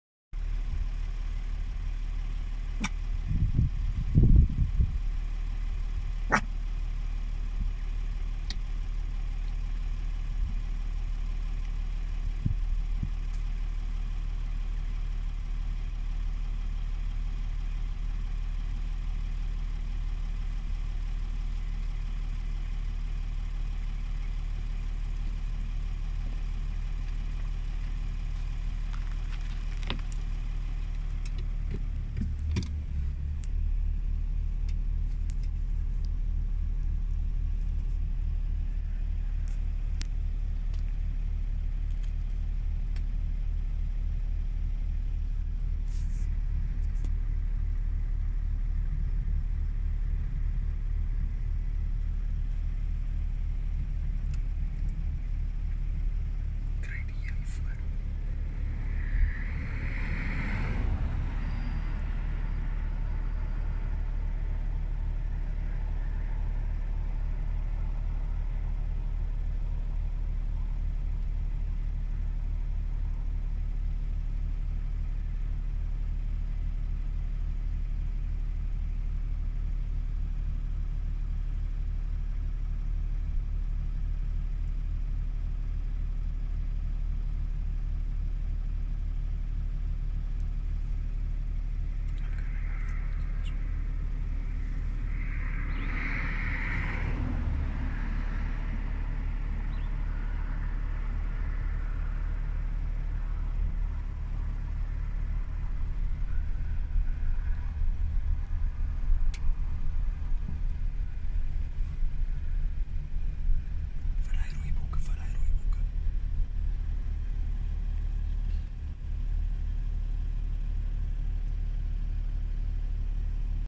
Addo lunch South Africa.
Giraffe 🦒 having lunch.